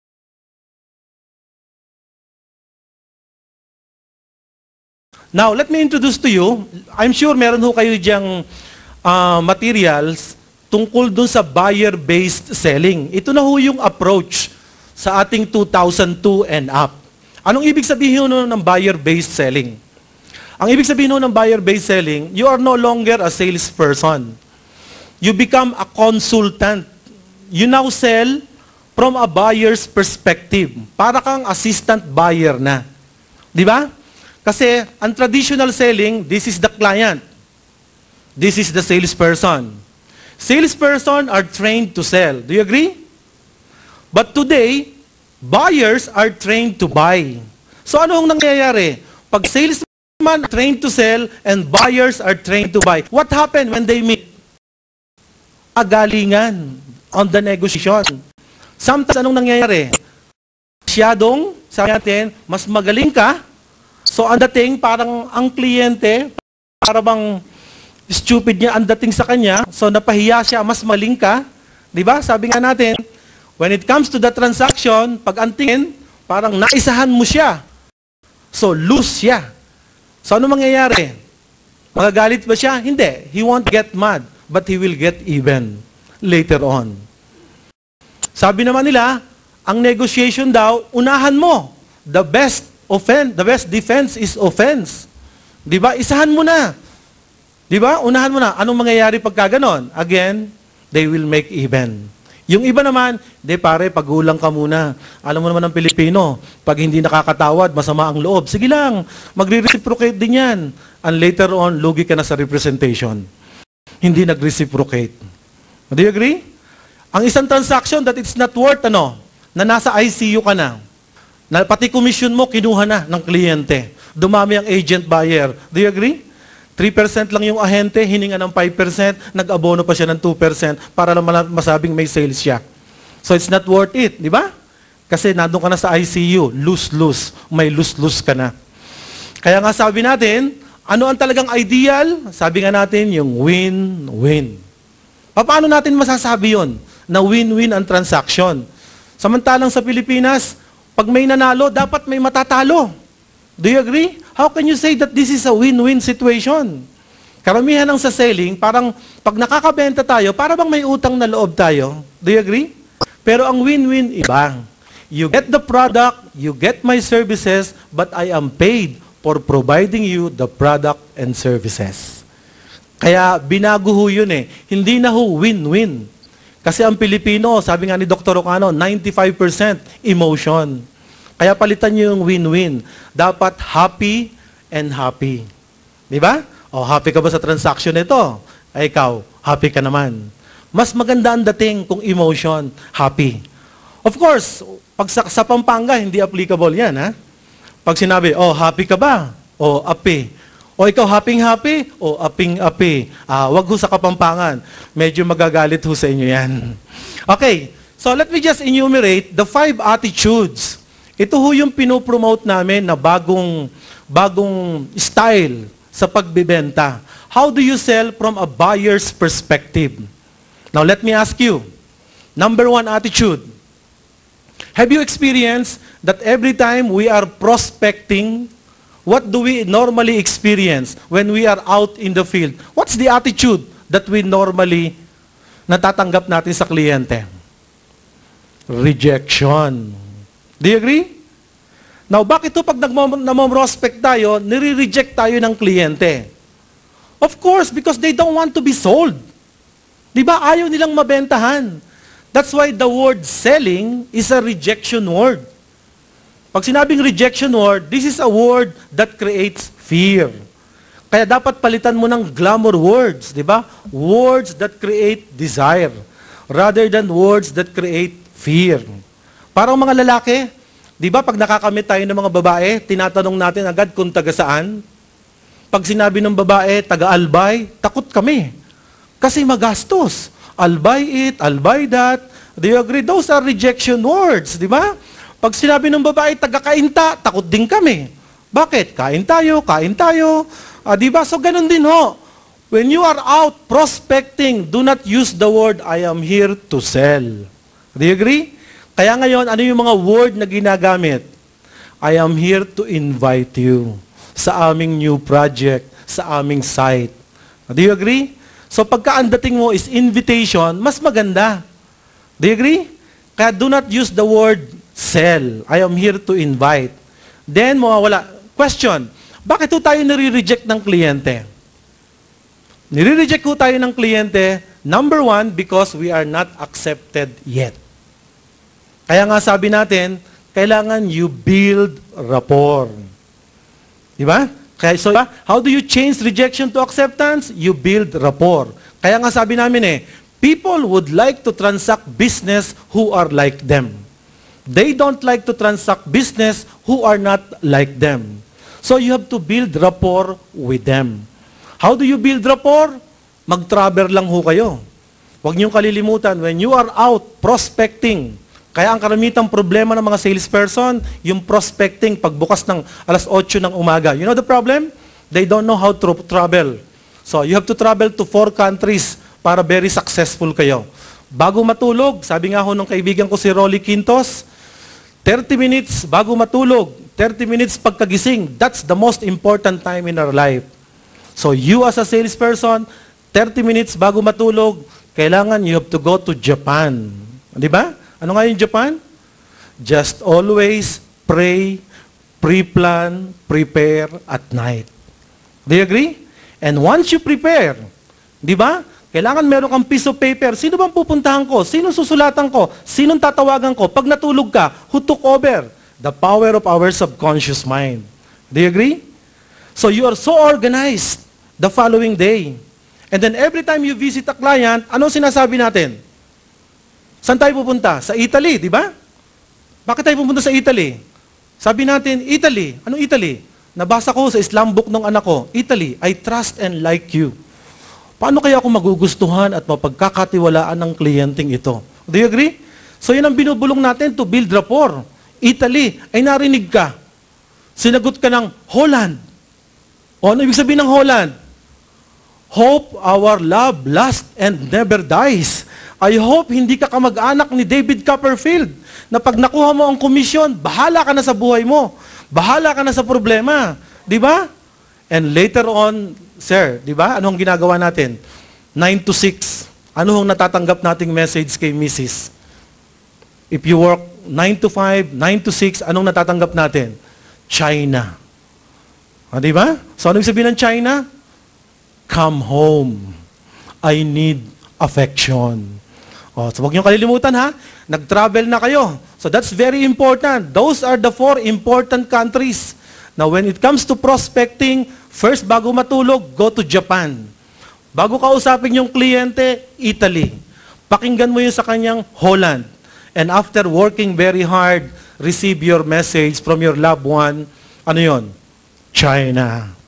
Part 4 on Buyer Based Selling - The Art of Breakthrough Selling by Mr. Jun Garing
PART 4 on Buyer Based Selling
Exsell 2002: Filipino Art of Breakthrough Selling by Mr. Jun Garing of Salesman's Centre & Train Pilipinas.